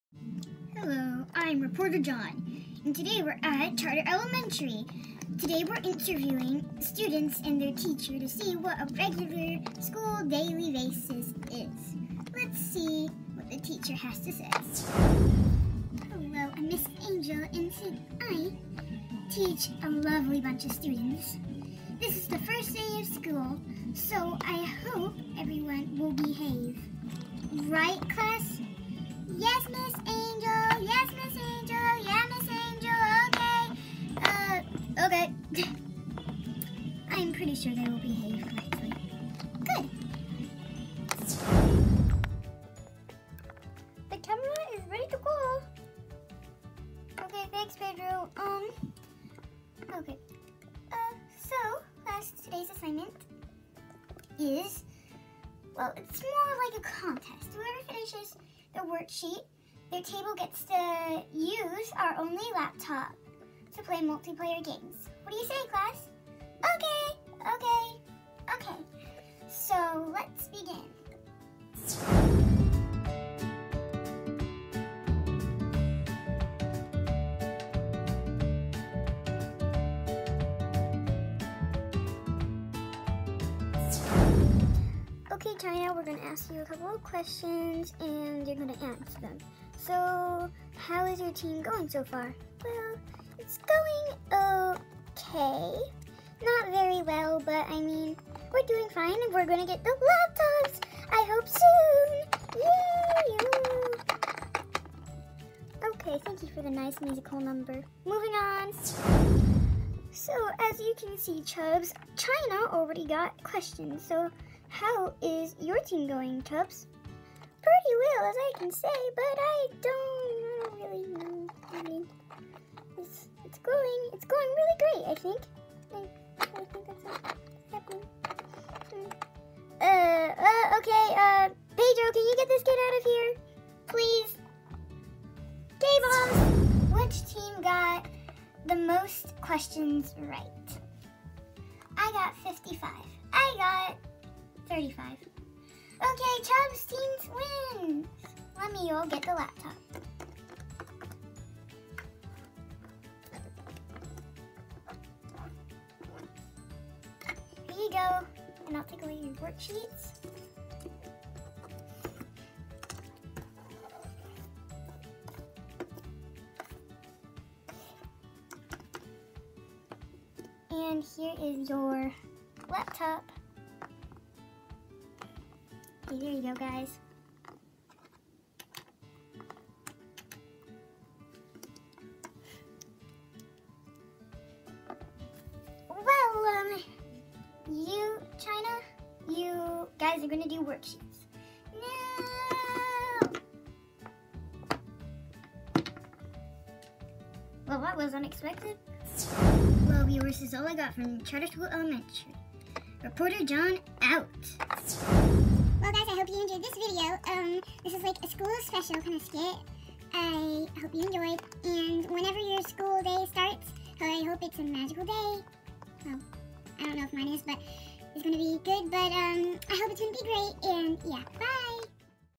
First day of school!
This where a group of reporters interview a class in a school on the first day.